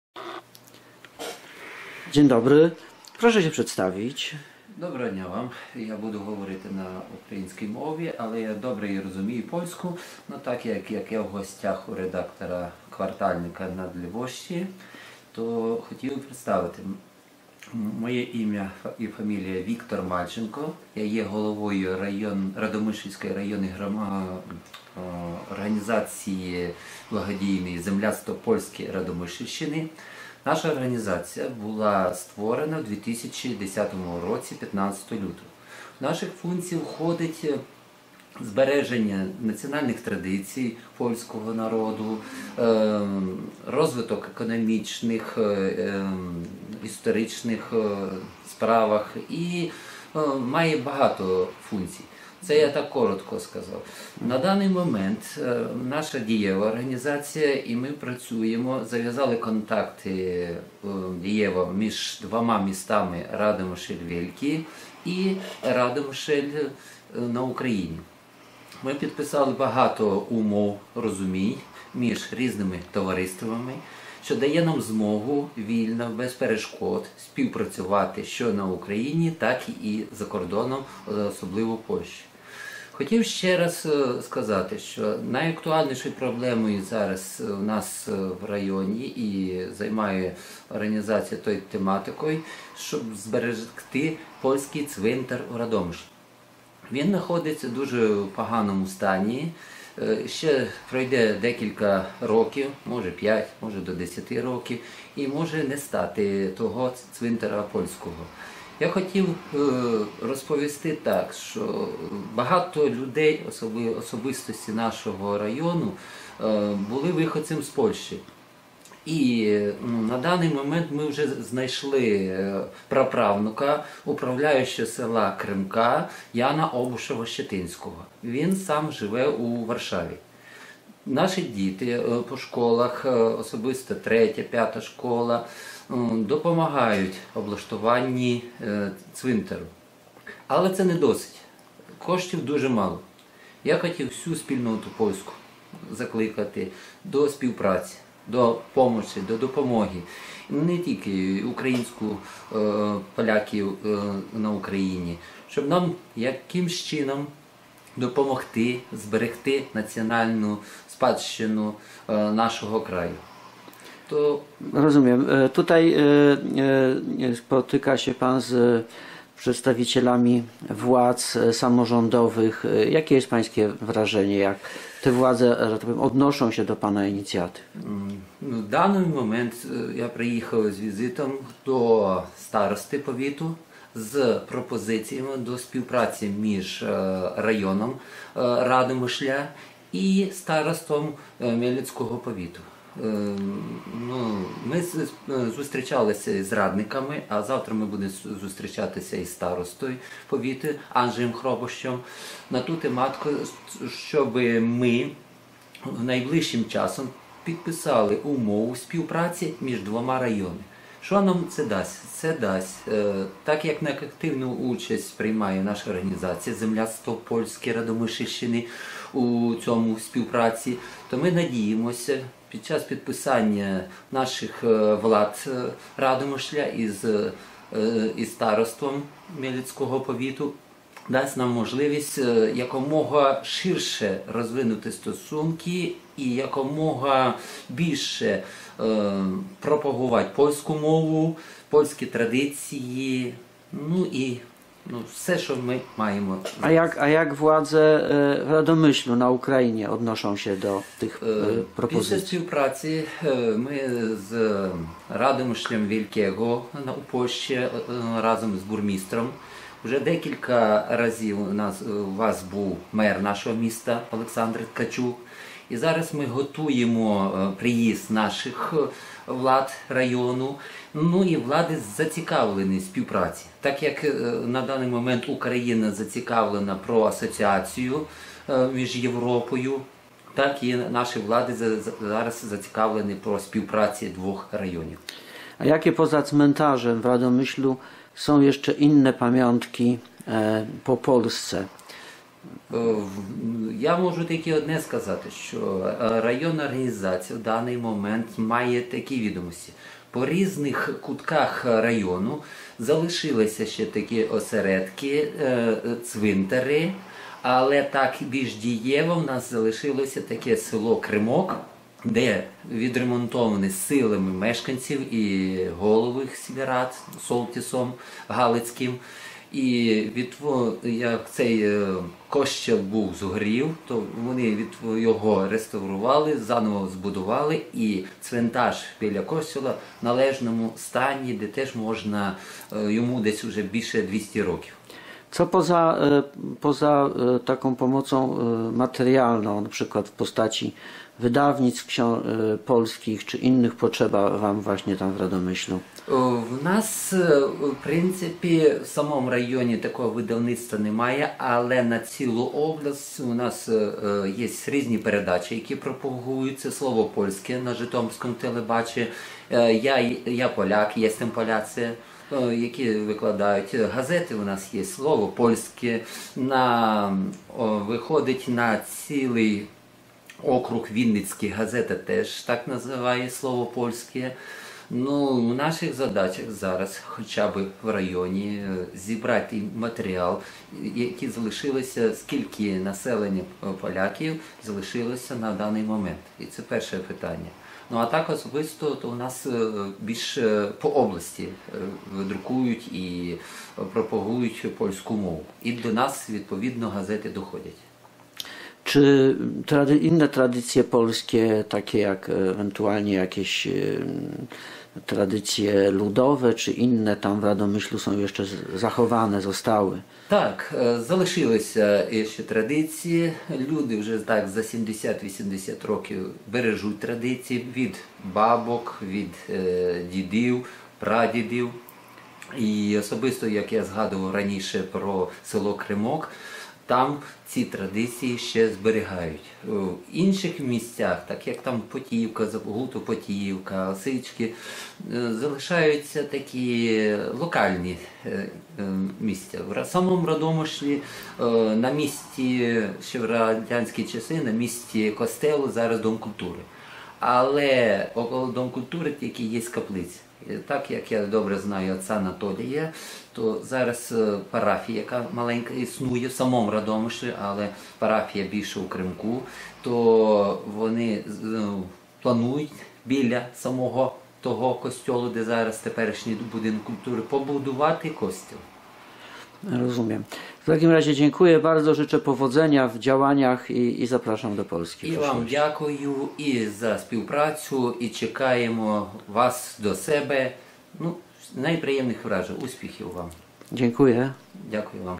Wiktor Malczenko - prezes Ziomkostwa Polskiego
Wiktor Malczenko - prezes Ziomkostwa Polskiego w Radomyślu na Ukrainie, który jest miastem partnerskim Radomyśla Wielkiego w Polsce, w powiecie mieleckim. Niedawno powstałe Ziomkostwo Polskie w Radomyślu na Ukrainie stara się dbać o polskie ślady w tym mieście i jego okolicy. Wiktor Malczenko wraz z innymi osobami, w tym ukraińską młodzieżą szkolną, porządkują stary cmentarz polski w Radomyślu. Poszukuje też potomków osób, które są tam pochowane. Niedawno odnaleźli jednego z nich. Więcej informacji na naszej stronie